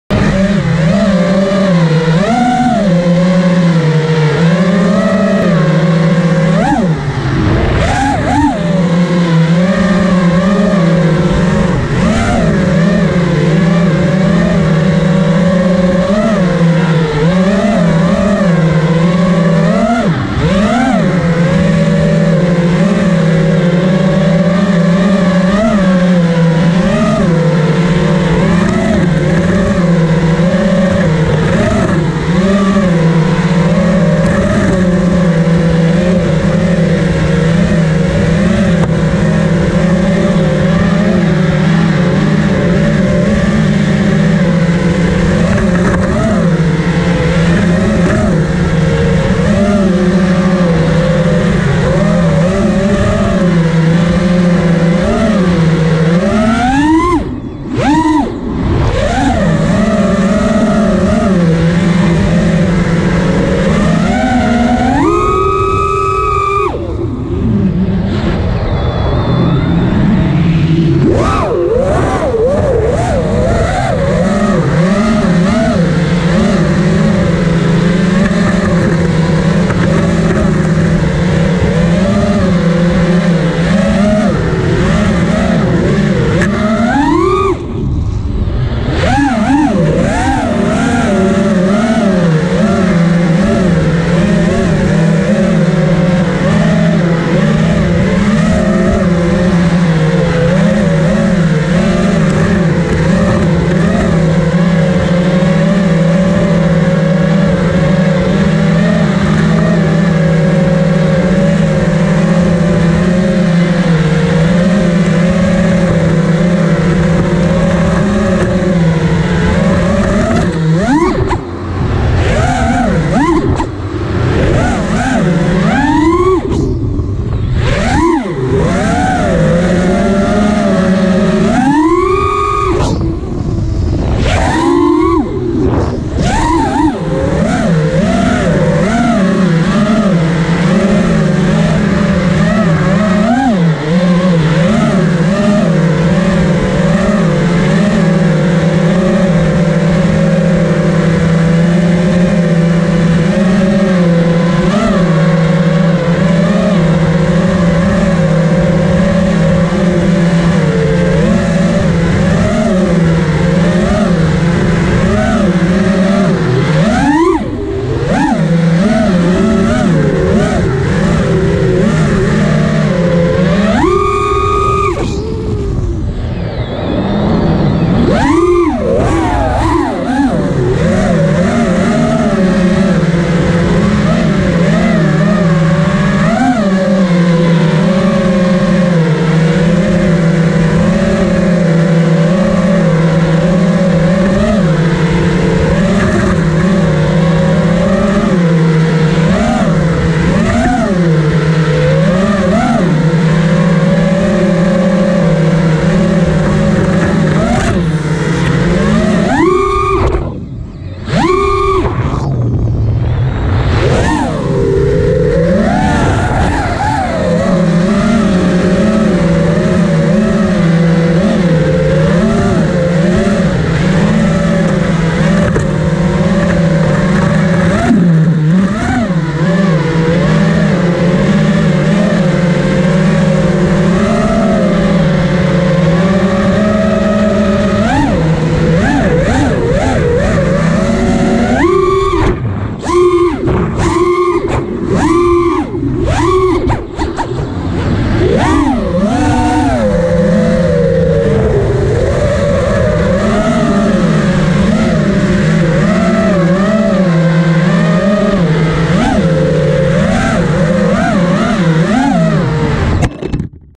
Chasing a human through tall grass with a drone
Just as finished flying this abandoned bit of waste land on edge lane, i came across lovely humans filming for a fine art degree right next to me.

I had one battery left so offered to do some filming for them.

The brief was. Get some chase footage and some shots from as low down as possible.